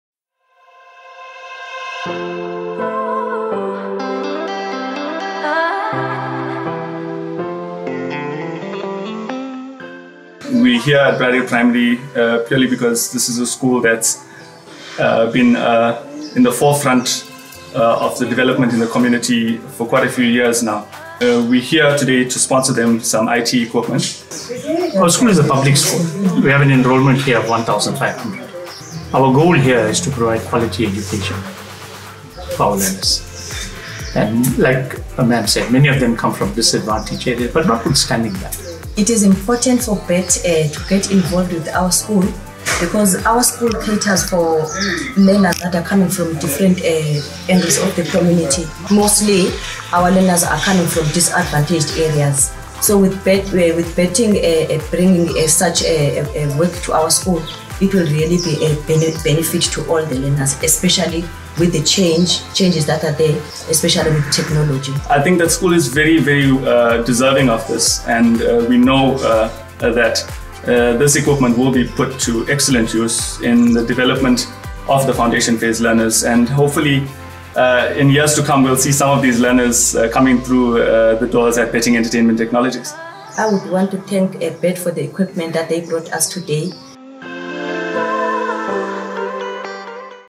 Briardale Primary School
We were proud to support Briardale Primary School by donating 5 desktop computers and contributing towards a brighter future in technology for all students!